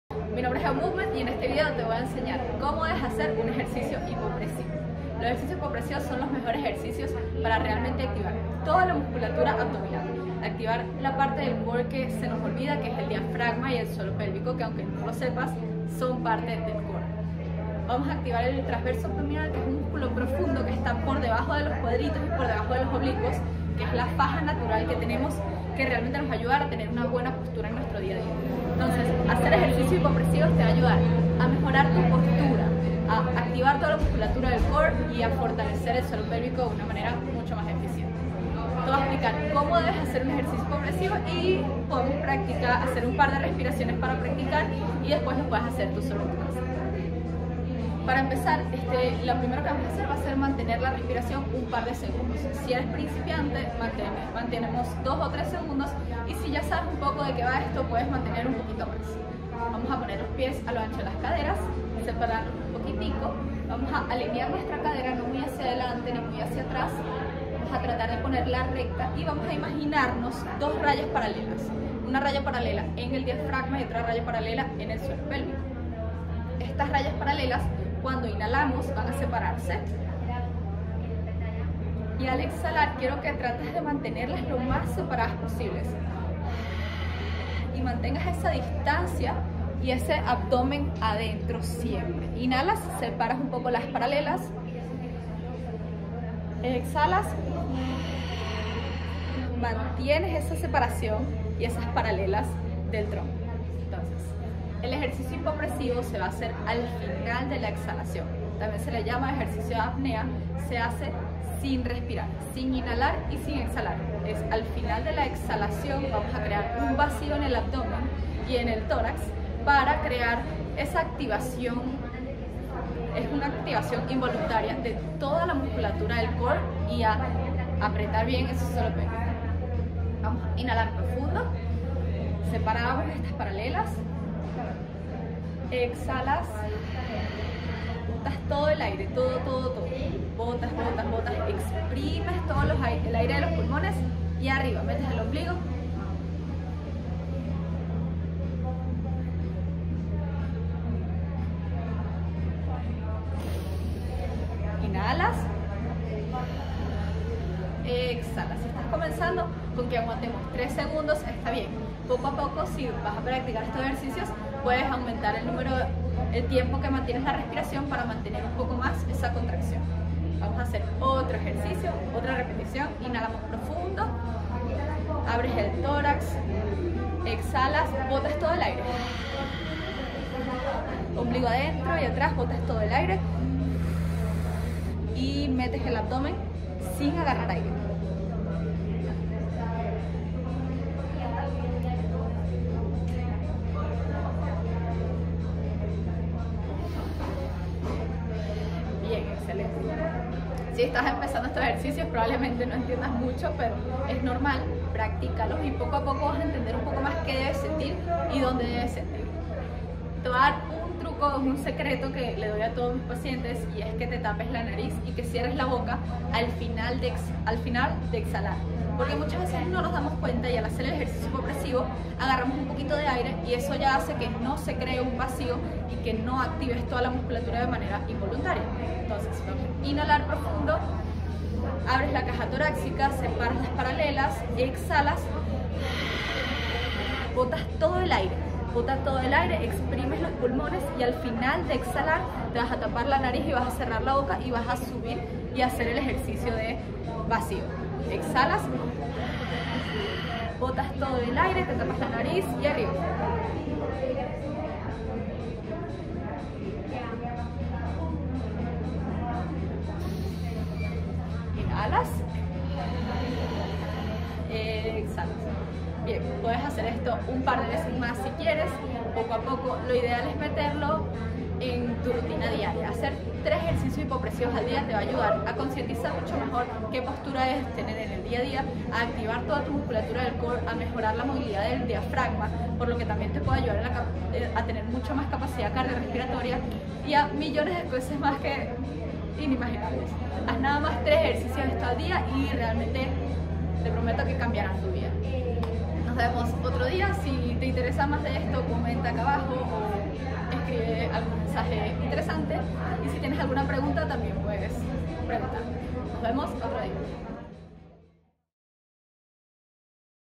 Ejercicios hipopresivos para principiantes con Eugenia Eseverri
Eugenia Eseverri, Fisioterapeuta y Health Coach, nos enseña cómo hacer los ejercicios hipopresivos y comparte un truco para que se te haga más fácil y puedas realizarlos correctamente. Ella recomienda incorporarlos en tu rutina diaria y hacerlos unas 3 veces en el día.

Los ejercicios hipopresivos te ayudarán a:
✨ activar toda la musculatura abdominal
✨ mejorar la postura
✨ fortalecer el suelo pélvico de forma eficiente

Momentos
00:00 Beneficios de los hipopresivos
00:47 Cómo hacer un hipopresivo
03:30 Truco para hacer hipopresivos